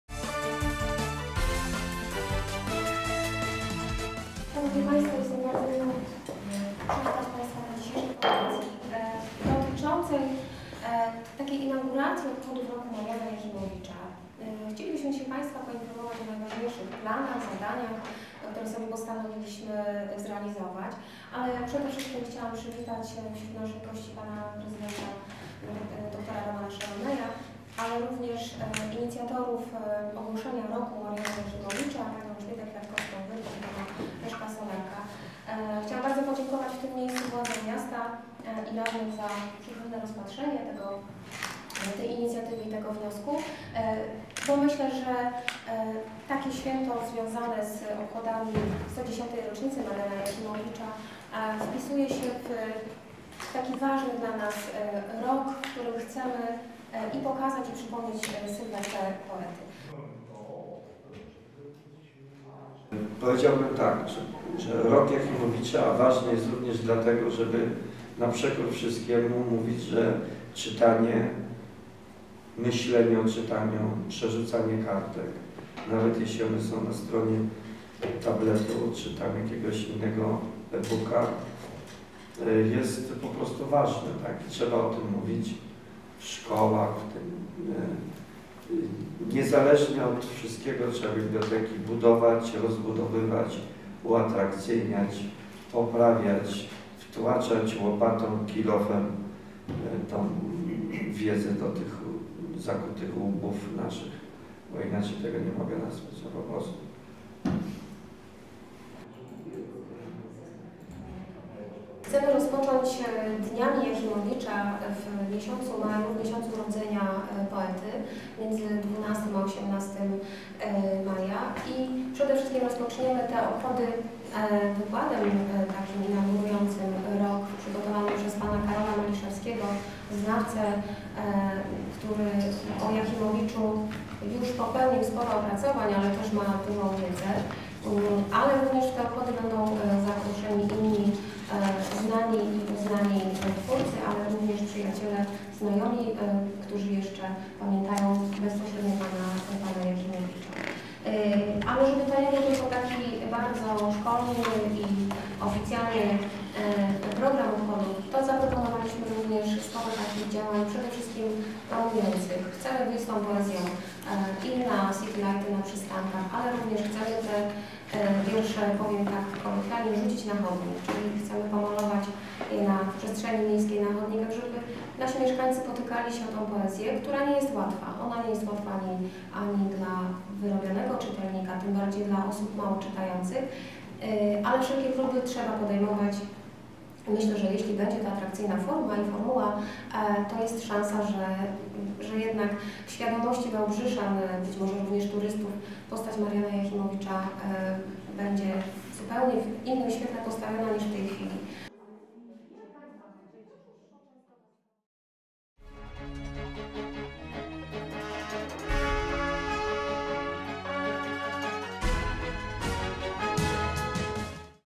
Rok Mariana Jachimowicza w Wałbrzychu
Ten rok będzie poświęcony cenionemu poecie Marianowi Jachimowiczowi związanego z Wałbrzychem . O bogatym programie jubileuszu 110. urodzin poety i promocji czytelnictwa na konferencji prasowej 17 marca opowiadali m.in. prezydent Wałbrzycha Roman Szełemej oraz dyrektor Biblioteki pod Atlantami Renata Nowicka.